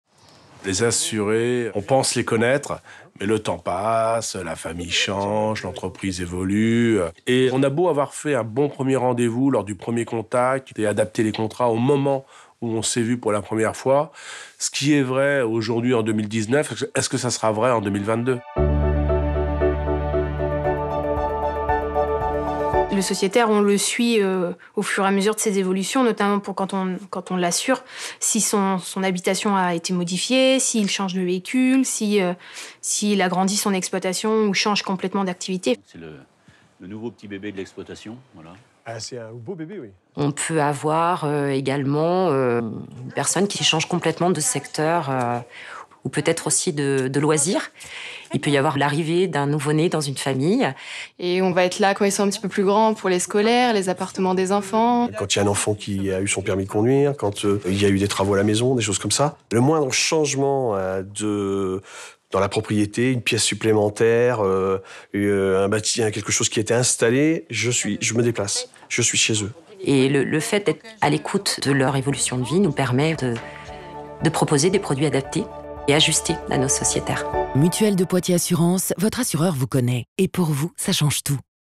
Mutuelle de Poitiers Assurances - Entretenir une relation durable
La communication vraie par Augural / Strateo avec Mutuelle de Poitiers Assurances

Défendre la communication vraie signifie présenter une approche du réel à ses clients afin de révéler les marques sans fard ni artifice, telles qu’elles sont vraiment. Voici l'une des vidéos réalisées dans le cadre de notre collaboration avec Mutuelle de Poitiers Assurances :

Entretenir une relation durable